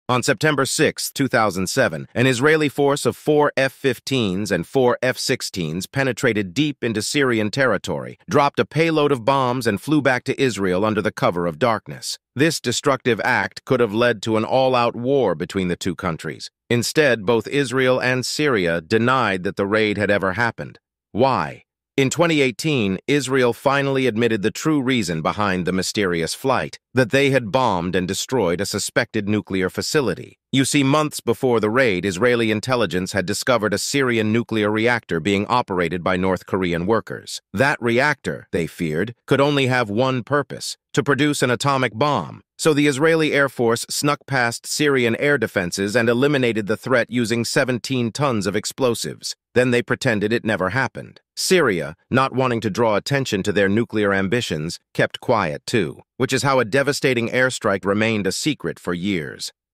When Israel Bombed a Syrian Nuclear Facility and Kept It a Secret 🤔
In September 2007, the Israeli Air Force flew into Syria, dropped a payload of bombs and then flew back to their base in Israel. Strangely, both Israel and Syria kept the incident under wraps for years… But in 2018 Israel finally revealed the real reason behind the attack — to knock out a Syrian nuclear facility.